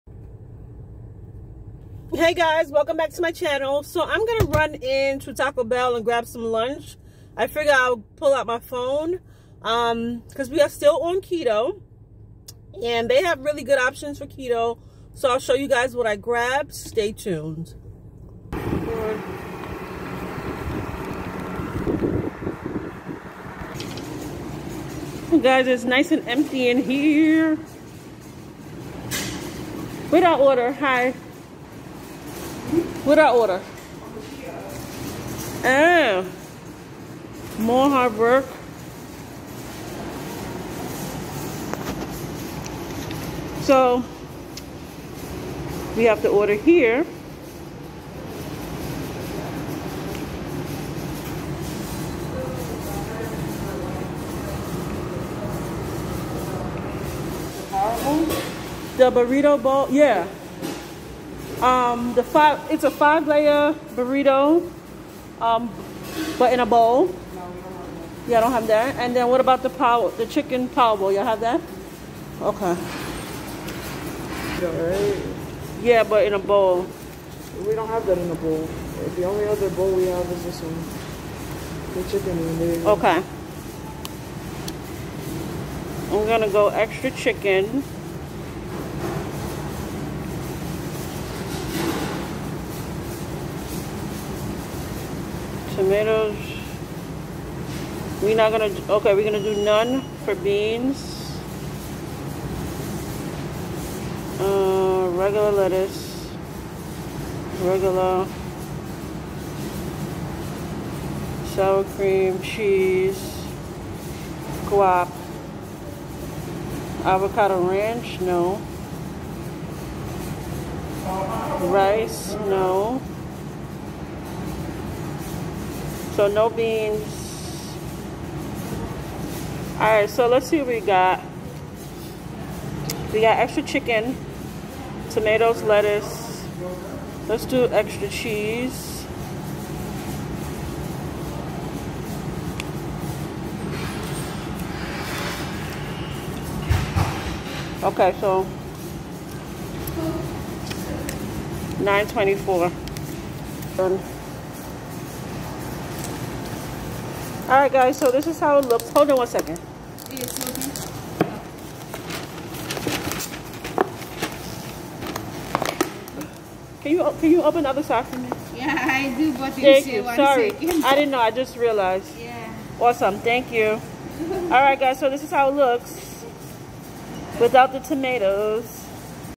How to order Keto @ Taco 🌮 Bell!
Follow me inside while  I show you how I order Keto @ Taco Bell ! They didn't have the 5 layer Burrito Bowl !